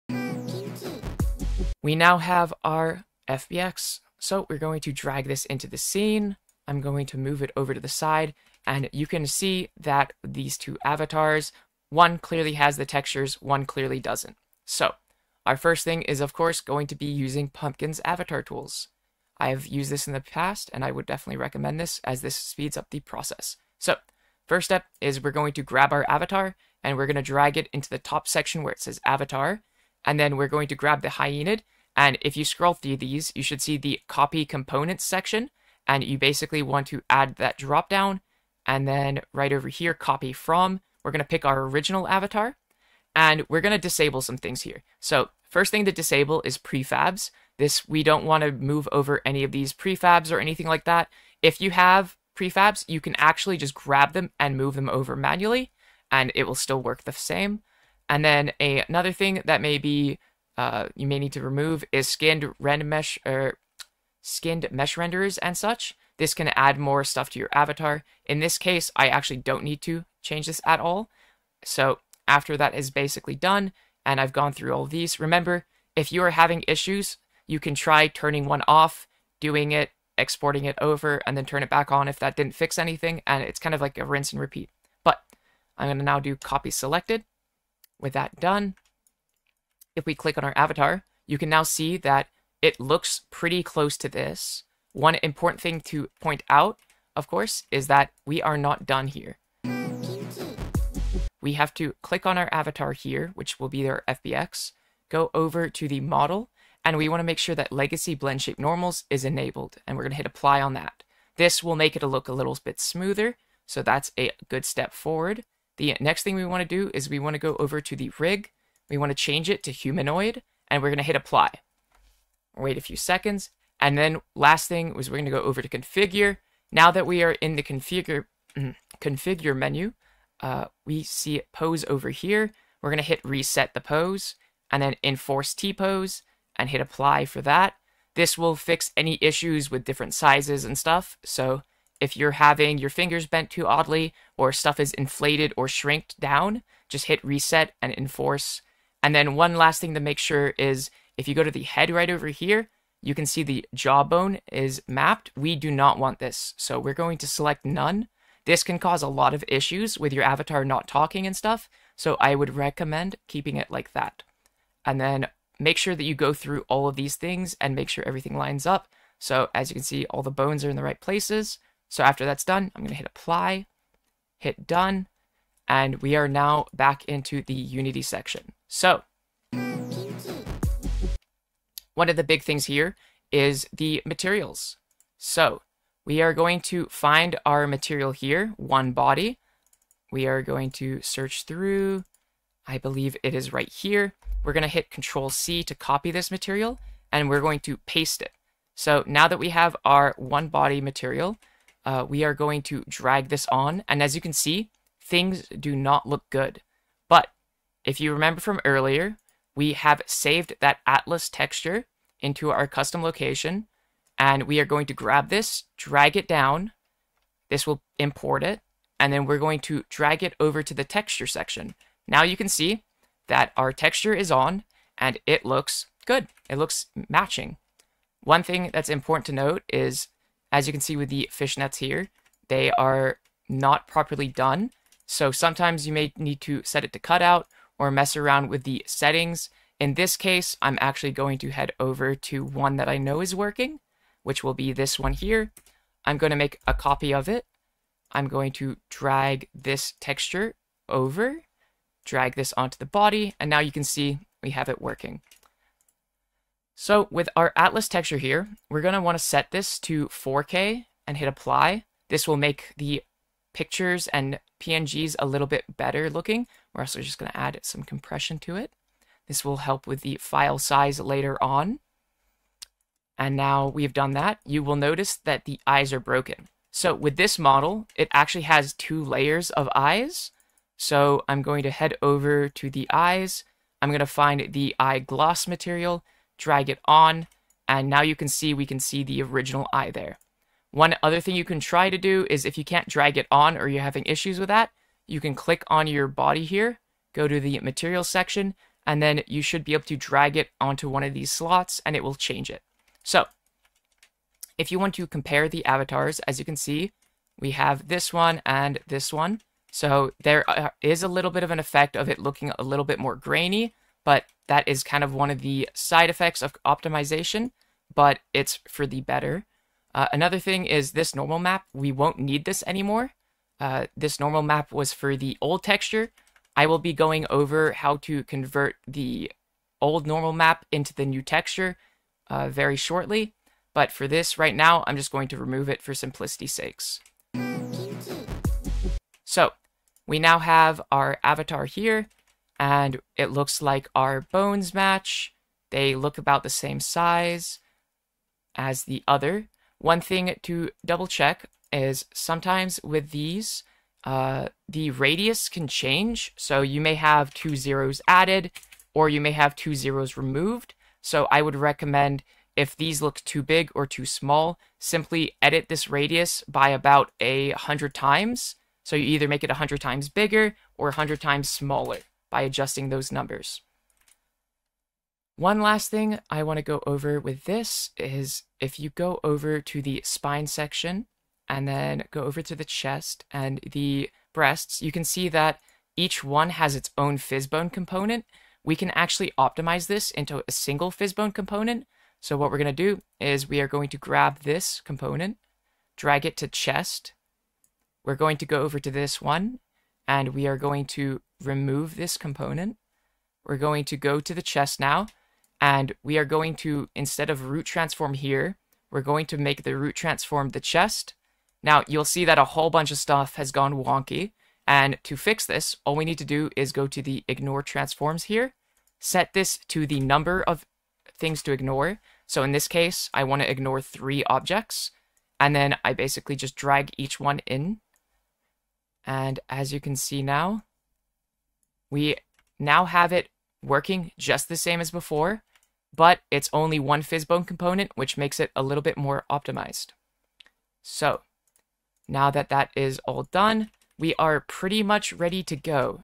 [2/4] VRChat Optimization Tutorial (Pumpkins Avatar Tools)
This is the second of multiple parts to my full optimization guide!

In this video, I go over Pumpkins Avatar Tools and the rigging menu within Unity! I also go over an optimization tip with VRChats Physbones components!
TIMESTAMPS
0:00 - Part 3: Pumpkins Avatar Tools
1:39 - Part 3.1: Unity Rig Menu
2:59 - Part 3.2: Setting up Materials/Textures
5:36 - Part 3.3: Physbone settings/Tips
TOOLS:
Show your support!